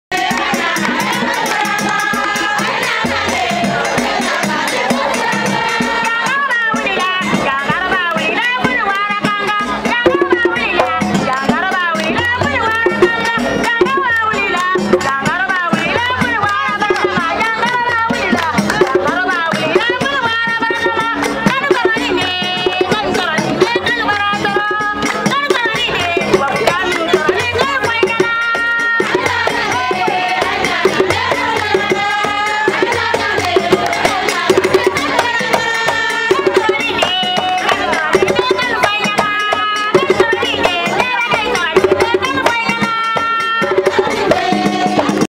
inauguration école de Téré 300
Guinée Préfecture de Kankan, Sous Préfecture de Sabadou Baranama.

Inauguration de l'école de Téré financée par Non Nobis, les femmes griottes chantent . 2013